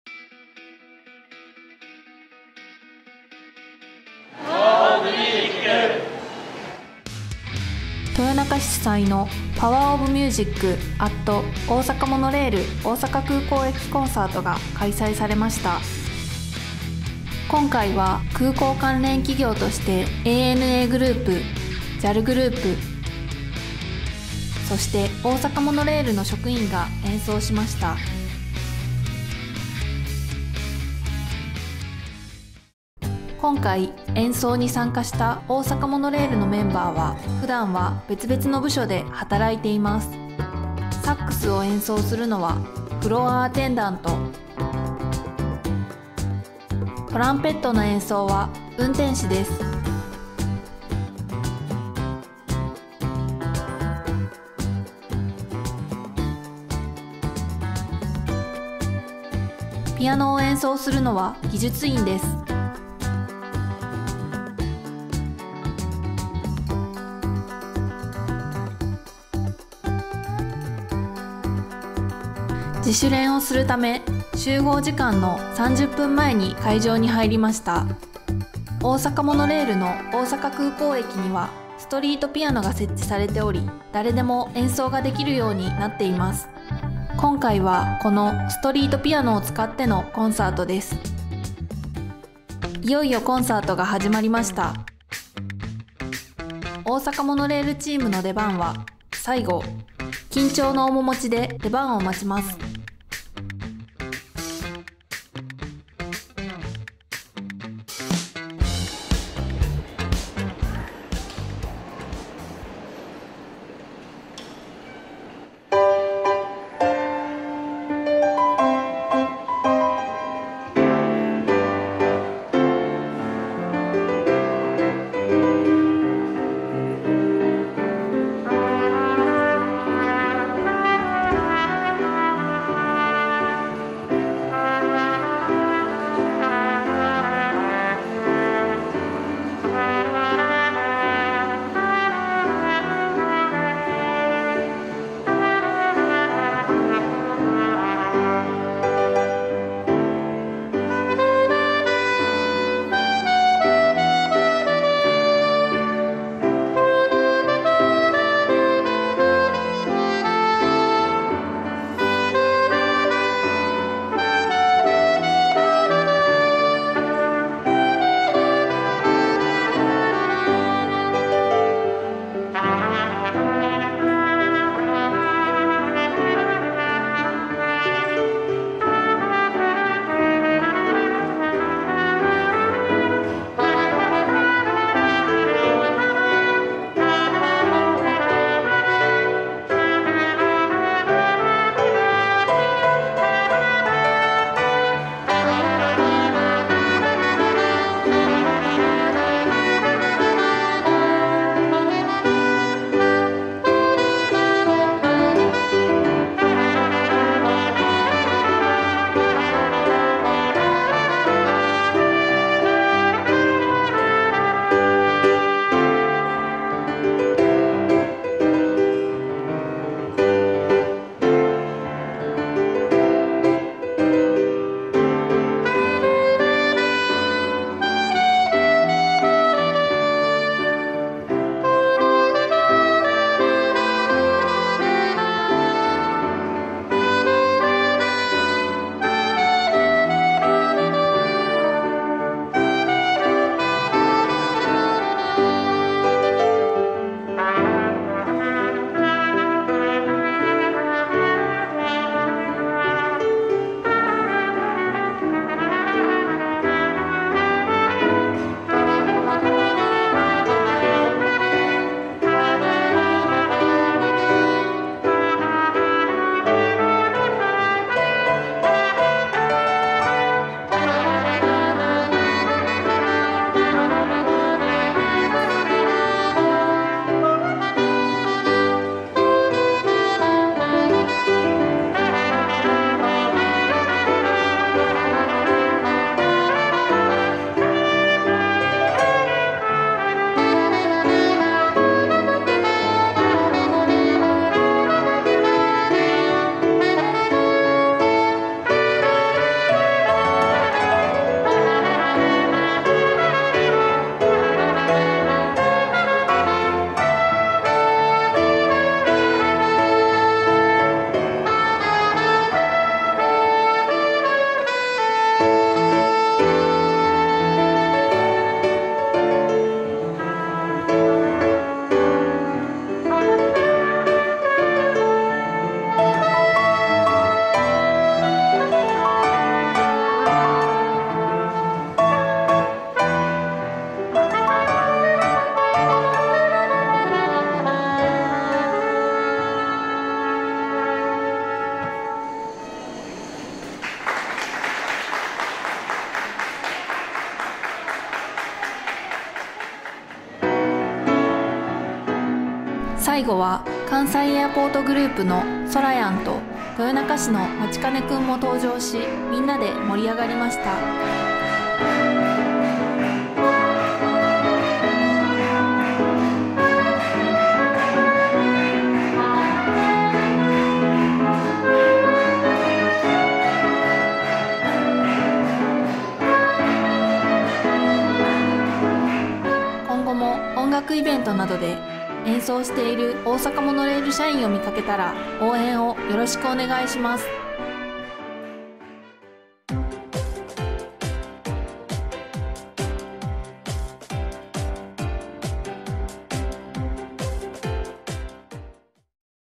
# POWER OF MUSIC ＠大阪モノレール大阪空港駅コンサート 2023年3月24日【公式】
2023年3月24日、大阪空港駅構内にて# POWER OF MUSIC ＠大阪モノレール大阪空港駅コンサートが開催されました。

JALグループ、ANAグループの職員と大阪モノレールからは運転士、フロアアテンダント、技術員が演奏しました。演奏の様子をぜひお楽しみください！